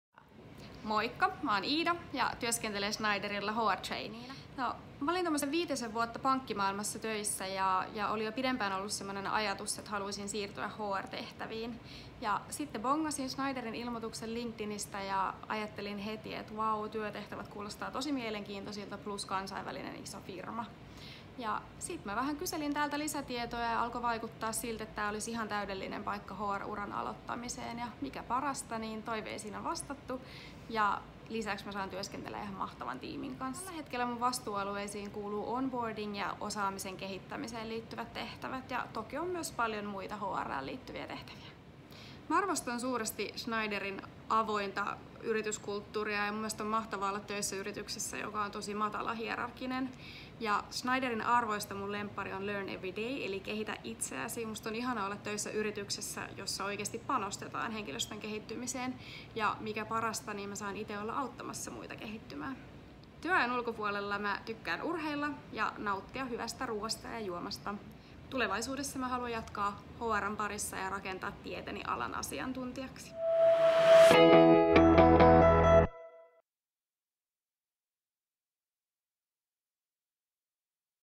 Finanssimaailmasta HR uralle | Schneider Electric
HR Trainee Iida Pajunen kertoo miten siirtyi finanssimaailmasta HR uralle Schneider Electriciin ja millaisia vastuita hänellä on.